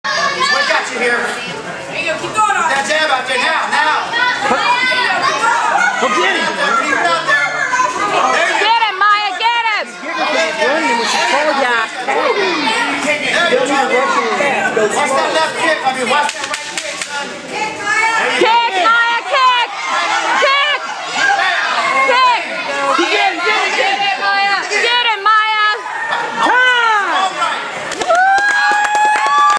RSKA Cage Wars 2011 Maiyah End of Round 3 Fight 4
The last thirty seconds of her last fight of the day.